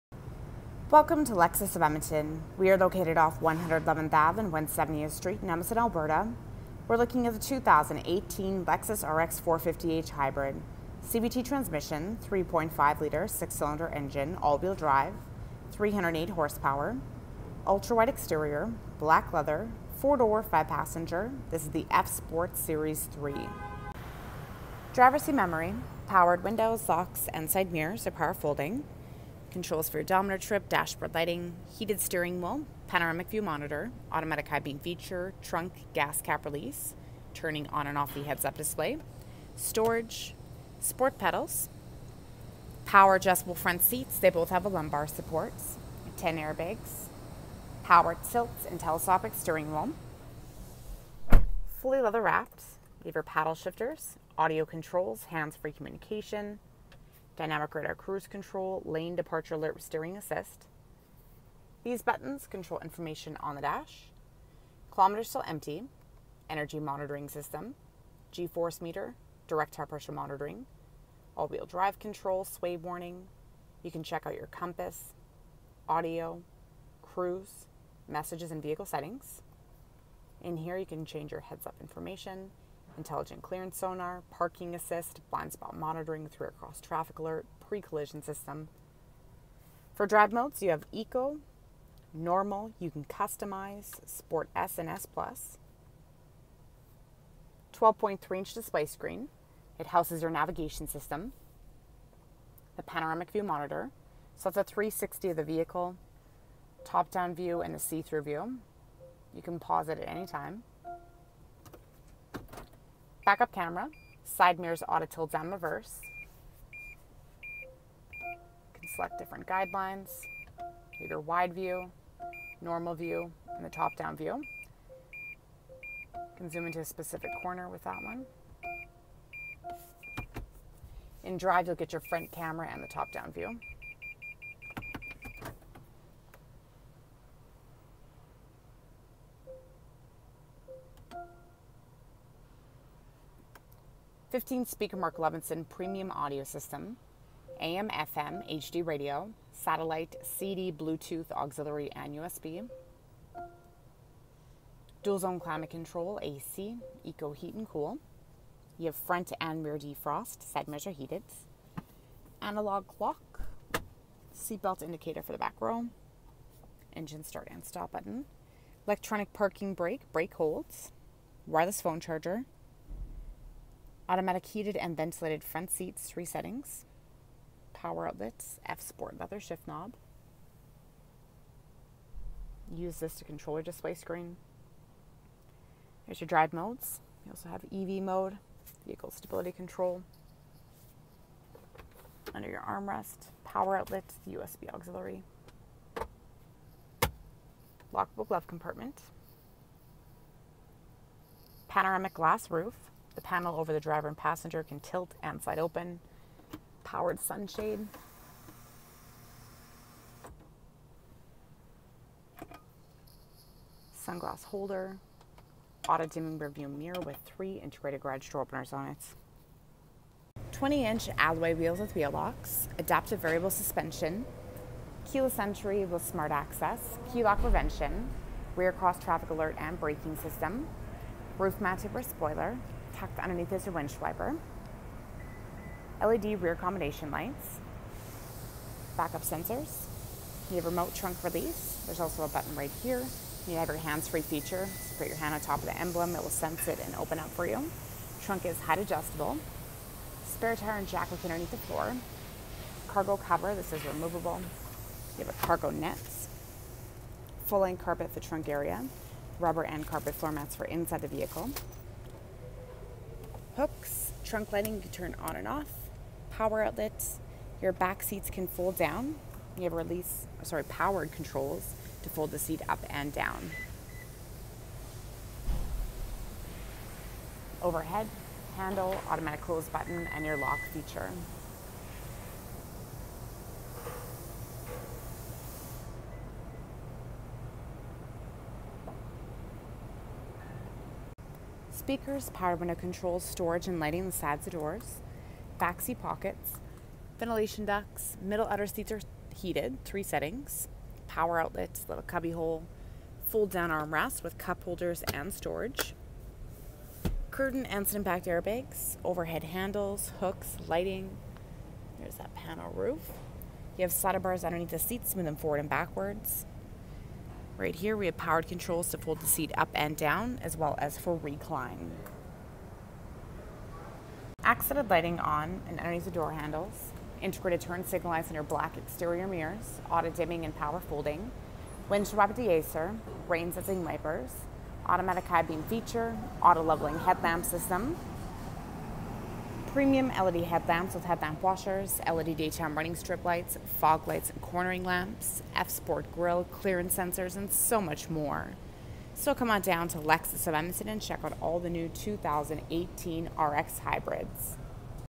White 2018 Lexus RX 450h F Sport Series 3 Walkaround Review West Edmonton Alberta
This is a 2018 Lexus RX 450h AWD with CVT Transmission White color and Black interior color.  (Uploaded by DataDriver).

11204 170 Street
Edmonton, Alberta
T5S 2X1